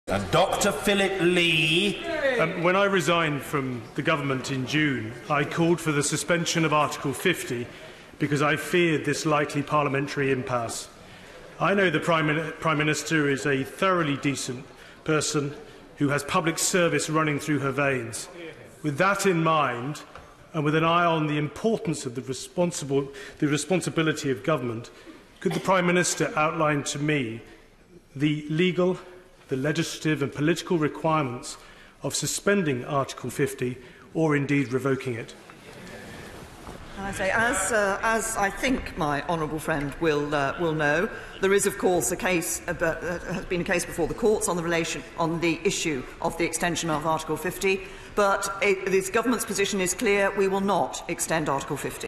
Brexit statement - Question to the Prime Minister
Question on revoking or suspending Article 50